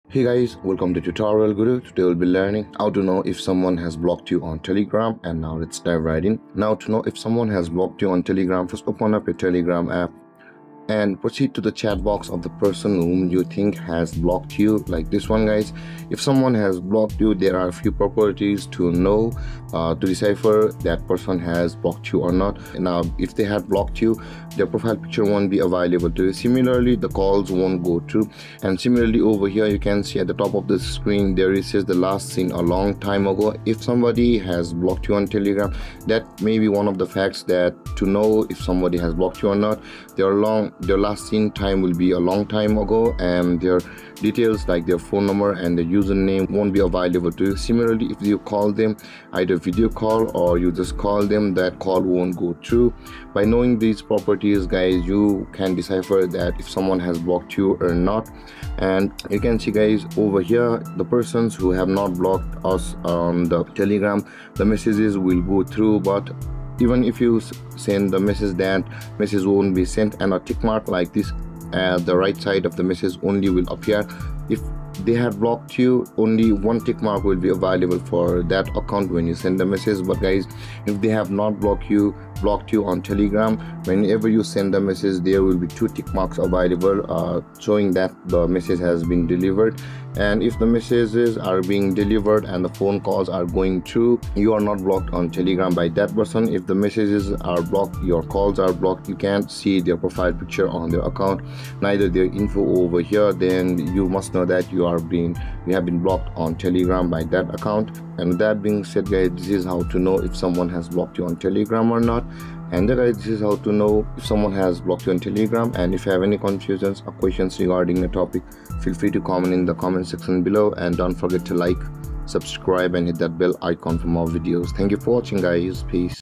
How to Know If Someone Has Blocked You on Telegram 2024?
Welcome to TutorialGuru! Wondering if someone has blocked you on Telegram in 2024? In this video, we'll show you how to easily find out if you've been blocked on Telegram. Whether you're using Telegram on your phone or desktop, this tutorial covers the steps to help you determine if someone has blocked you.